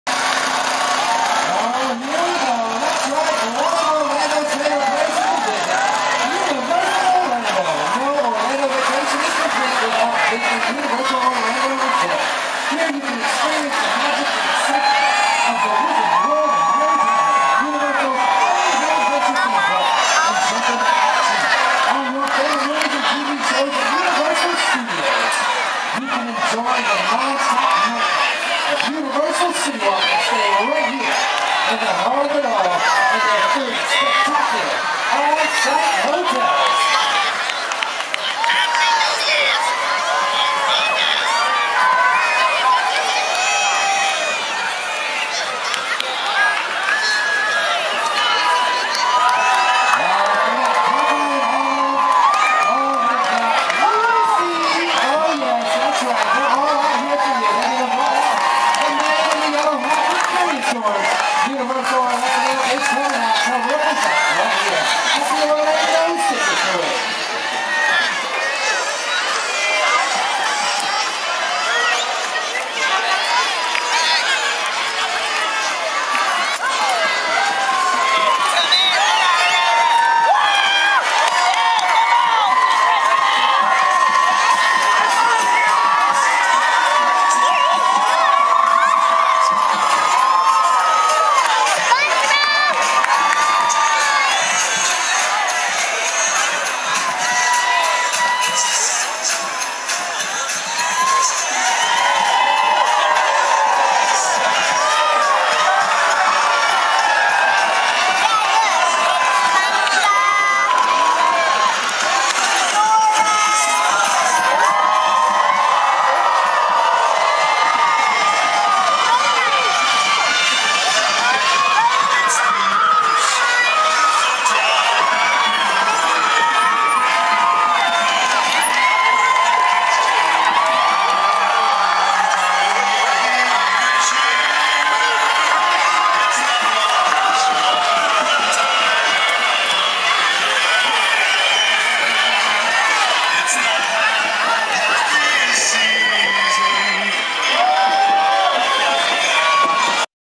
Florida Citrus Parade 2010: Universal Orlando Resorts Characters
Orlando Citrus Fresh From Florida Parade 2010:  Universal Orlando Characters.  Live event